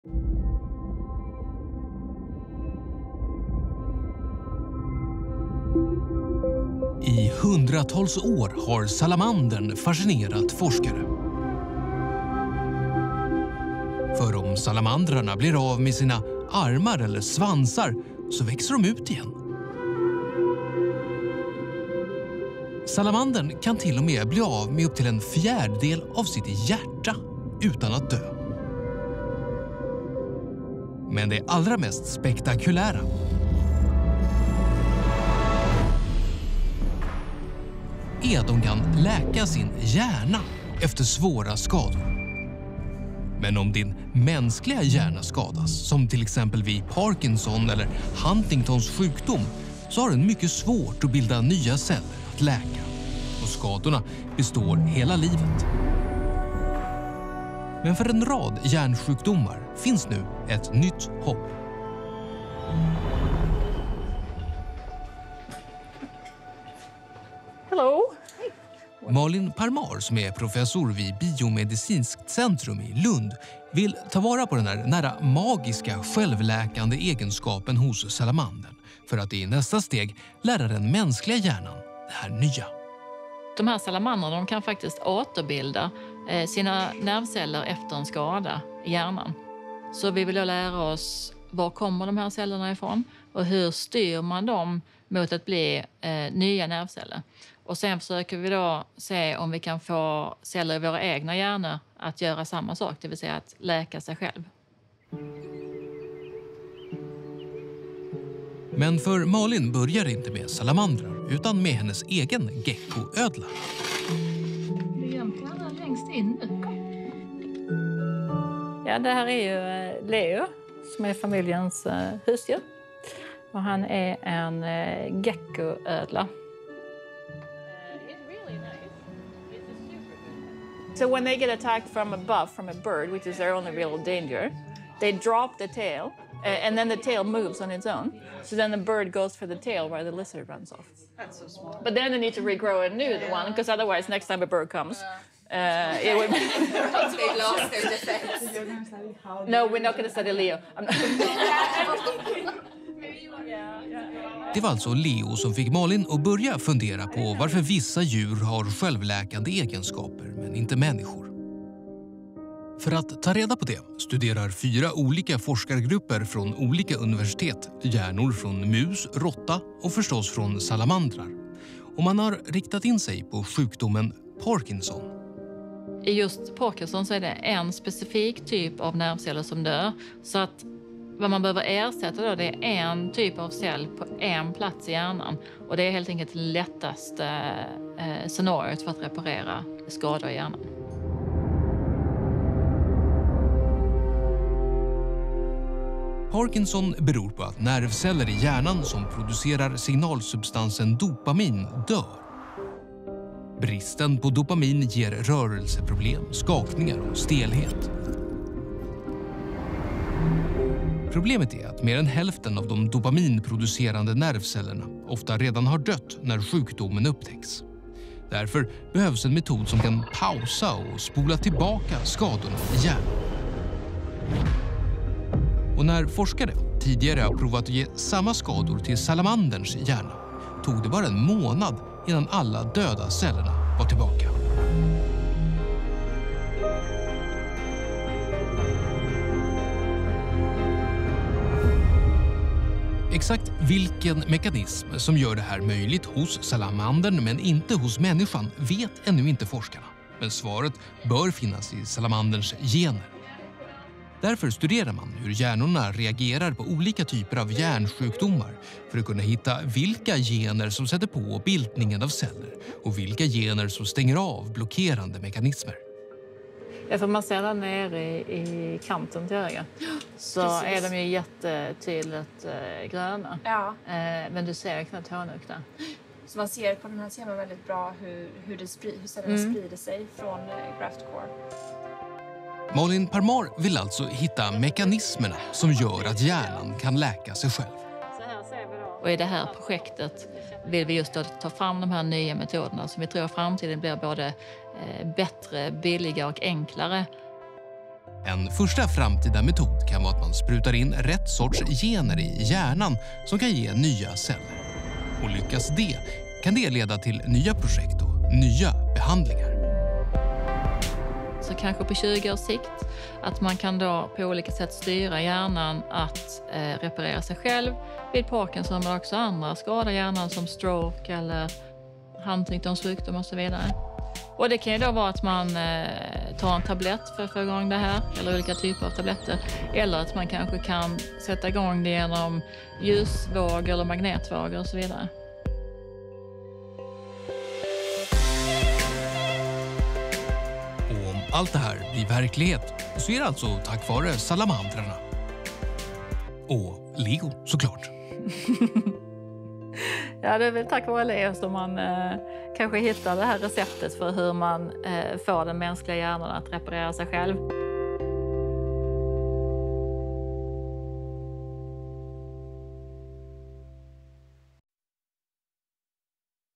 Salmandrar ger hopp om ny behandling av Parkinson
Salamandrar har en förmåga att självläka. Om de blir av med en arm eller en svans så växer den ut igen. Det mest spektakulära är att de också kan läka sin hjärna efter svåra skador, genom att återbilda sina nervceller. Malin Parmar, professor vid Lunds universitet, vill förstå processen för att hitta nya behandlingar för bland annat Parkinsons och Huntingtons sjukdom.